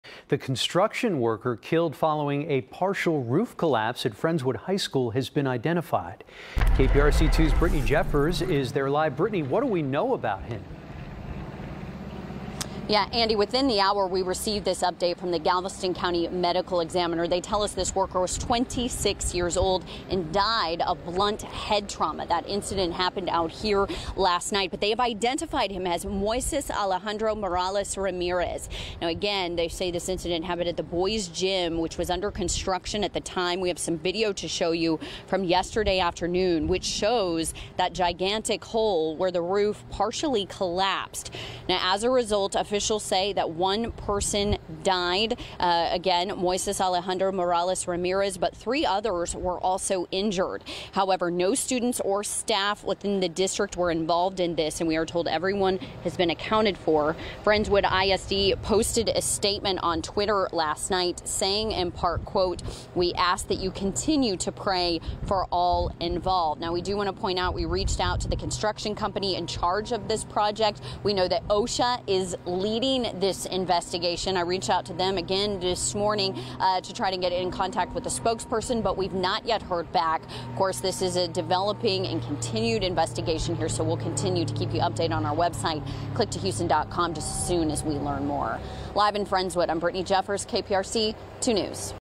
26-year-old construction worker killed after roof collapse at Friendswood High School identified
A 26-year-old construction worker killed after roof collapse at Friendswood High School was identified on Wednesday.